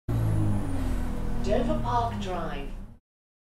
Dover Park Drive
iBus announcement recorded on route 493 (SE191) on the 06/12/18